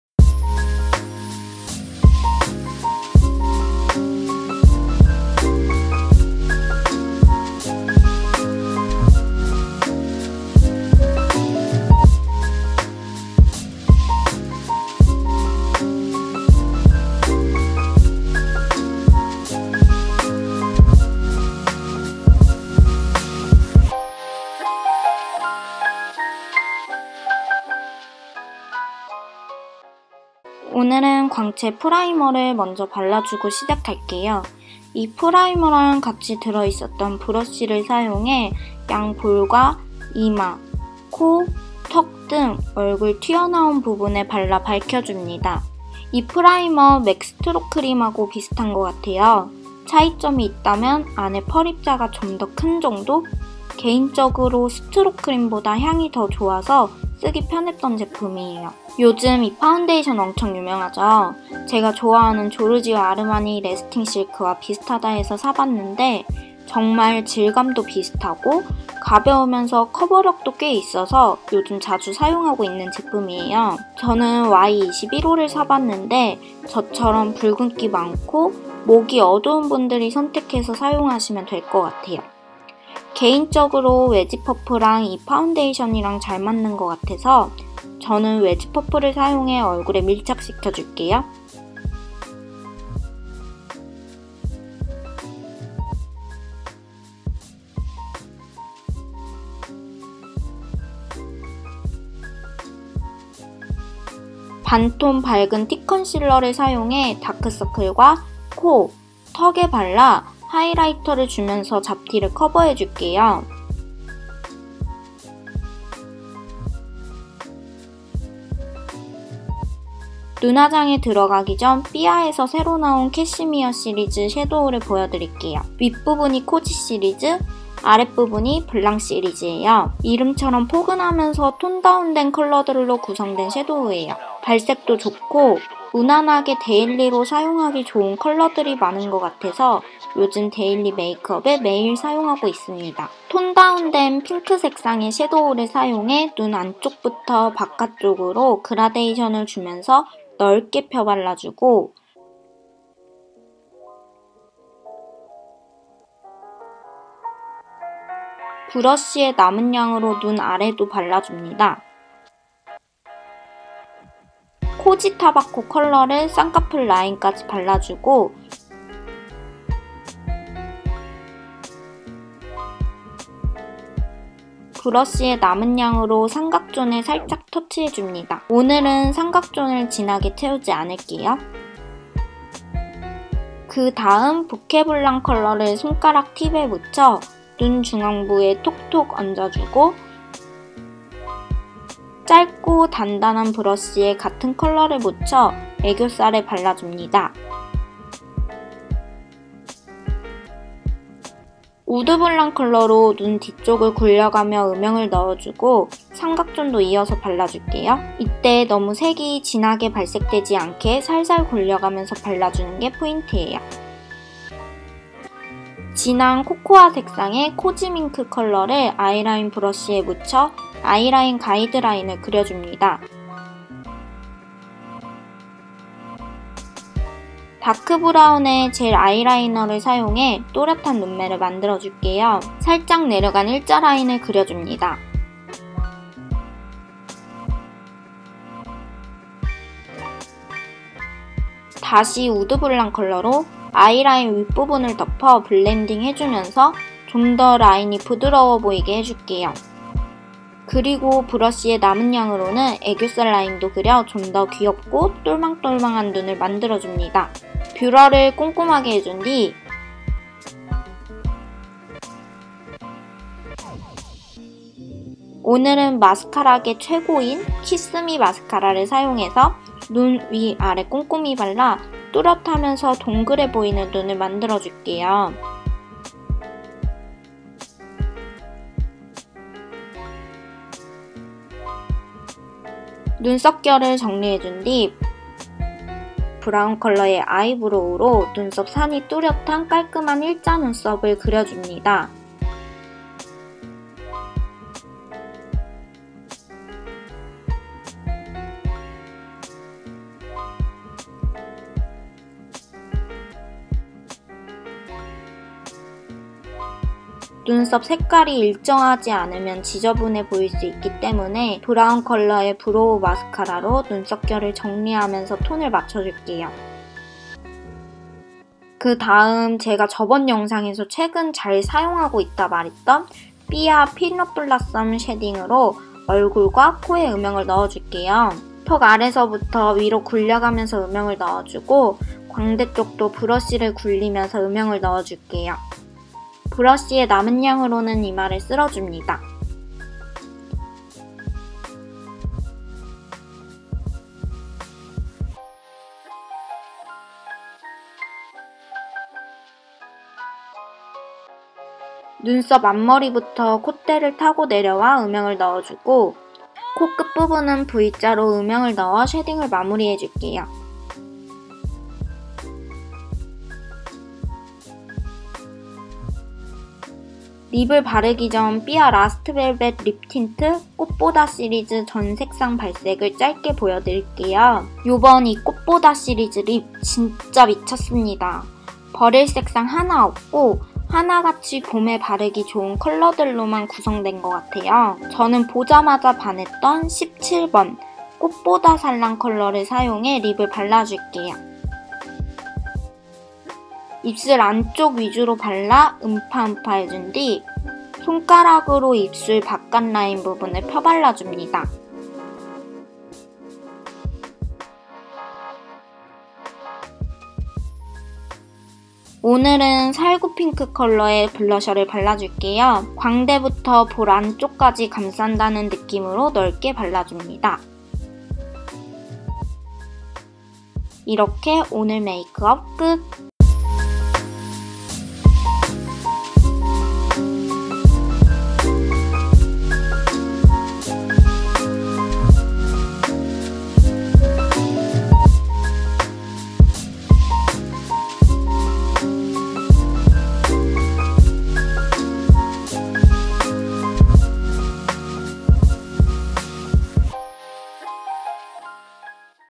[ENG] 삐아 봄시리즈로 해본 데일리 인간 벚꽃 메이크업🌸(cherry blossom makeup) ⎮ HIGH
하이~여러분
오늘은 벚꽃축제 가기 전 찍었던
벚꽃메이크업을 찍어 봤어요!
아침에 해가 뜨고 있는 상태에서 자연광 촬영을 하다보니
화면의 색감이 일정하지 않은 점 양해 부탁드려요 ㅠㅠ
아마.. 5월 쯤 벚꽃 브이로그가 올라가지 않을까 싶습니다...🌸
오늘도 영상 봐주셔서 너무 감사해요!

🐾사용한 제품

✻ 렌즈
프레쉬콘 모자익 (어반 그레이)

✻ 프라이머
코드
g.루미에르 프라이머 - 20,000won

✻ 파운데이션
네이처리퍼블릭 프로방스 에어 스킨 핏 원데이 래스팅 파운데이션 (y21 퓨어바닐라) - 18,000 won

✻ 컨실러
루나 롱래스팅 팁 컨실러 (01 라이트베이지) - 14,000 won

✻ 섀도우1
삐아 캐시미어 섀도우2 (08 로즈블랑) - 9,000 won

✻ 새도우2
삐아 캐시미어 섀도우1 (04 코지타바코) - 9,000 won

✻섀도우3
삐아 캐시미어 섀도우2 (06 부케블랑) - 9,000 won

✻ 섀도우4
삐아 캐시미어 섀도우2 (10 우드블랑) - 9,000 won

✻ 섀도우5
삐아 캐시미어 섀도우1 (05 코지밍크) - 9,000won

✻ 아이라인
프로에잇청담 스테이 온 젤 아이라이너 (02 브라운) - 23,000 won

✻ 마스카라
키스미 히로인메이크 롱앤컬 마스카라 - 18,000 won

✻ 아이브로우
네이처리퍼블릭 극세사 브로우 펜슬 (03 소프트브라운) - 7,000 won

✻ 브로우마스카라
아리따움 아이돌 브로우 마스카라 (02. 내추럴브라운) - 5,500 won

✻ 쉐이딩
삐아 라스트 블러쉬 (08 피넛블라썸) - 9,000 won

✻ 립
삐아 라스트 벨벳 립 틴트4 (17. 꽃보다살랑) - 12,000 won

✻ 블러셔
세잔느 내추럴치크 (10. 오렌지핑크) - 13,000 won

🐾product

✻ lens
freshkon mozaic (urban gray)

✻ primer
CODE
g.lumiere primer - 20,000won

✻ foundation
NATURE REPUBLIC
provence air skin fit one day foundation Y21 - 18,000 won

✻ concealer
LUNA
longlasting tip concealer #01 - 14,000 won

✻ shadow1
BBIA
cashmere shadow2 #08 - 9,000 won

✻ shadow2
BBIA
cashmere shadow1 #04 - 9,000 won

✻  shadow3
BBIA
cashmere shadow2 #06 - 9,000 won

✻ shadow4
BBIA
cashmere shadow2 #10 - 9,000 won

✻ shadow5
BBIA
cashmere shadow1 #05 - 9,000won

✻ eyeliner
PRO 8 CHENGDAM
stay on gel eye liner 02 brown - 23,000 won

✻ mascara
KISS ME
heroine make long & curl mascara - 18,000 won

✻ eyebrow
NATURE REPUBLIC
micro slim brown pencil # 03 soft brown - 7,000 won

✻ brow mascara
ARITAUM
idol brow mascara #02 - 5,500 won

✻ shading
BBIA
last blush #08  - 9,000 won

✻ lip
BBIA
last velvet lip tint4 #17  - 12,000 won

✻ blusher
CEZANNE
natural cheek #10 - 13,000 won